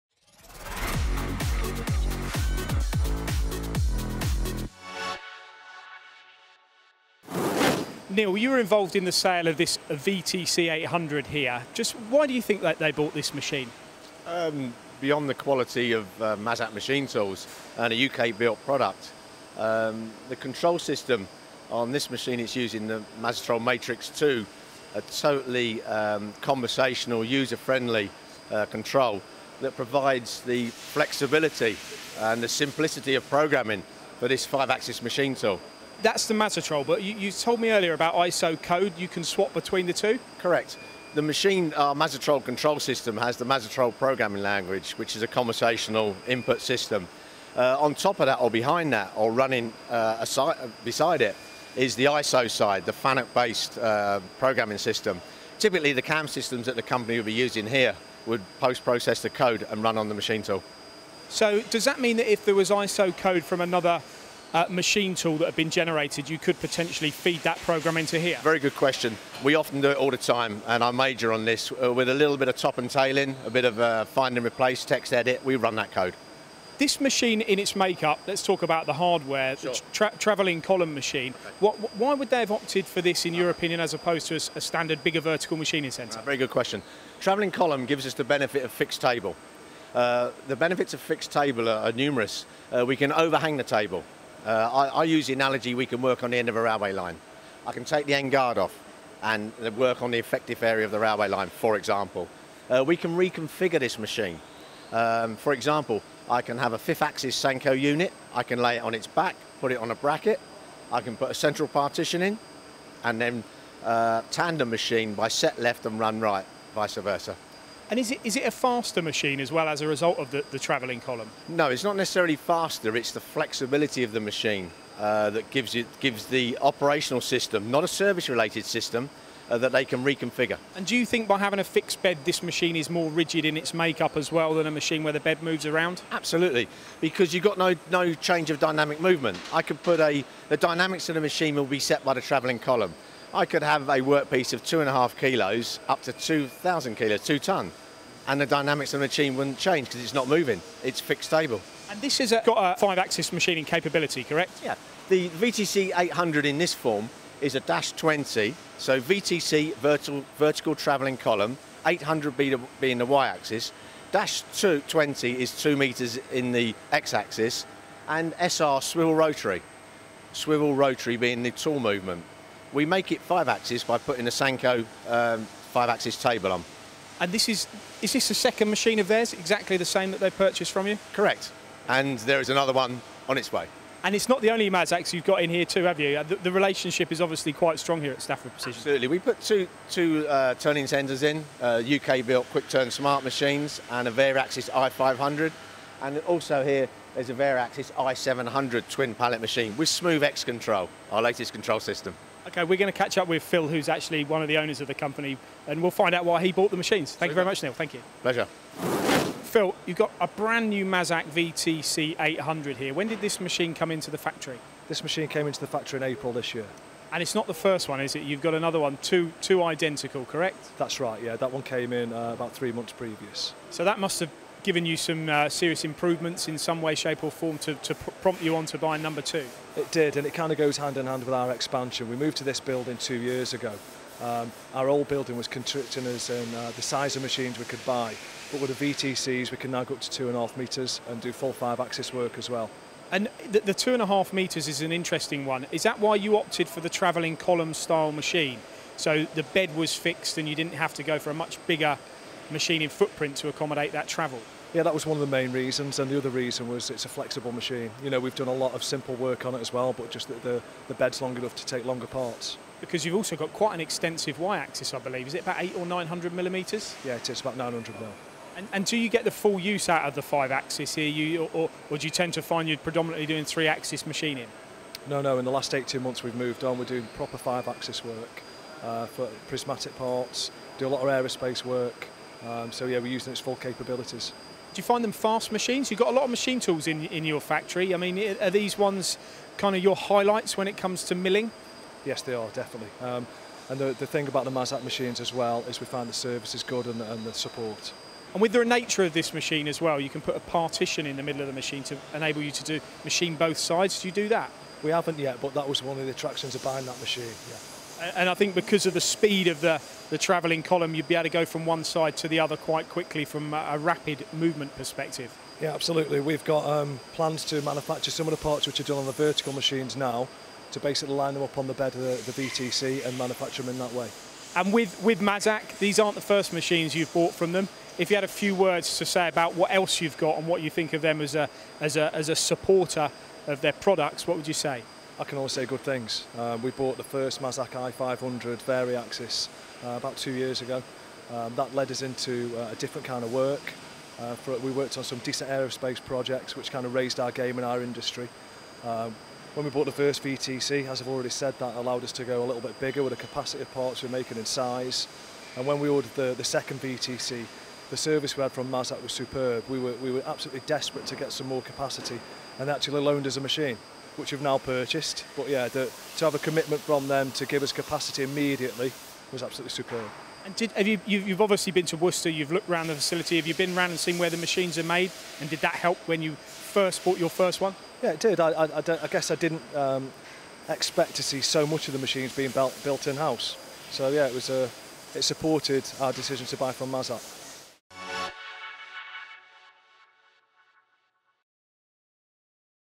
SPE buy 2 VTC 5 axis machines from Mazak
We talk with Phil Smith about his companies recent investment in two new 5 axis Vertical Travelling Column machine tools from Mazak. An interesting story about a company experiencing significant growth and opting for the latest, versatile machining technologies.